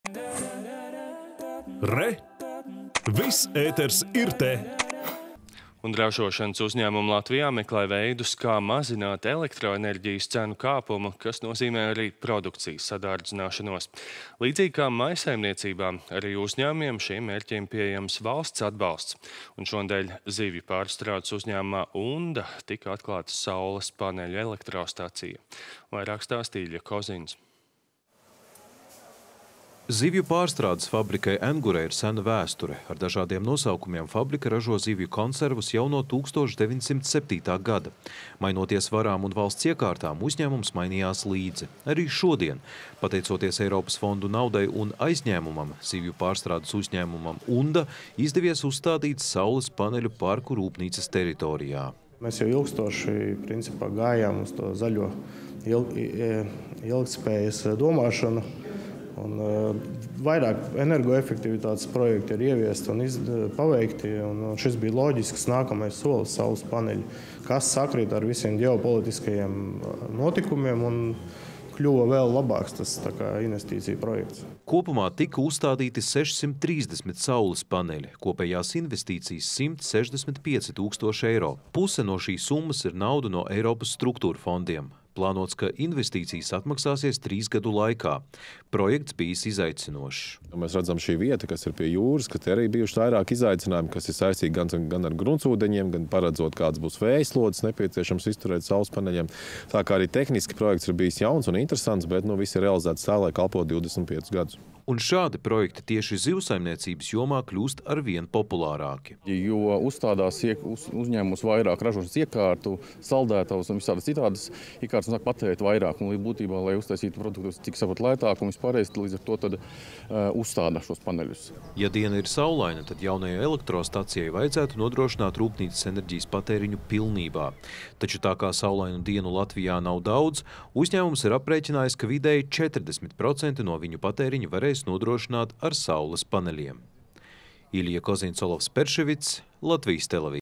Ar saules paneļiem mazinās ražošanas izmaksas
Ražošanas uzņēmumi Latvijā meklē veidus kā mazināt elektroenerģijas cenu kāpumu, kas nozīmē arī produkcijas sadārdzināšanos. Līdzīgi kā mājsaimniecībām, arī uzņēmumiem šiem mērķiem pieejams valsts atbalsts. Šonedēļ zivju pārstrādes uzņēmumā “Unda” tika atklāta saules paneļu elektrostacija. Stāsta Iļja Kozins.